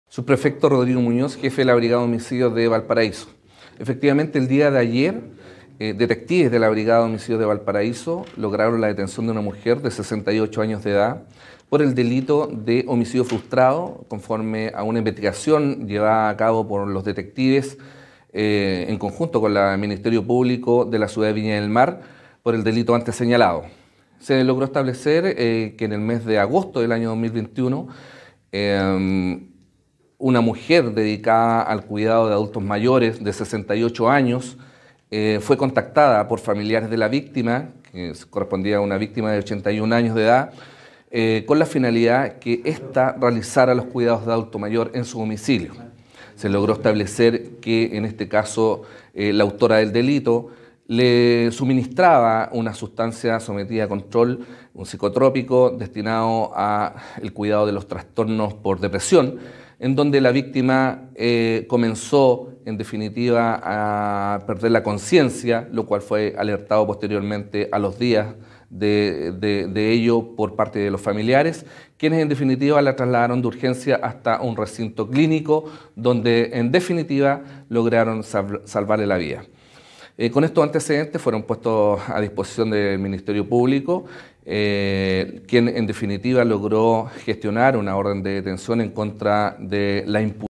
Mujer suministró farmácos adulto mayor para robar millonario botín en joyas en Viña del Mar
Detectives de la Brigada de Homicidios Valparaíso detuvieron a una mujer de 68 años, quien aprovechándose de su condición de cuidadora, le suministró fármacos a una adulta mayor con la finalidad de sustraerle especies avaluadas en 15 millones de pesos.

La mujer fue contratada el año pasado por familiares de la víctima de 81 años, a quien le dio en la alimentación importantes dosis de benzodiazepinas, ocasionándole pérdida de consciencia, para luego sustraerle diversas joyas y dinero en efectivo.

La familia trasladó a la adulta mayor a un centro asistencial, donde además de lograr su recuperación, le realizaron diversos exámenes detectando la presencia de la sustancia psicotrópica, de acuerdo a lo informado por el jefe de la Brigada de Homicidios Valparaíso, subprefecto Rodrigo Muñoz.

Conforme a las indagatorias que están en desarrollo y en coordinación con el Ministerio Público, se presume que existirían otras víctimas del mismo modus operandi de la imputada, quien fue capturada por la PDI y puesta a disposición del Tribunal de Garantía viñamarino.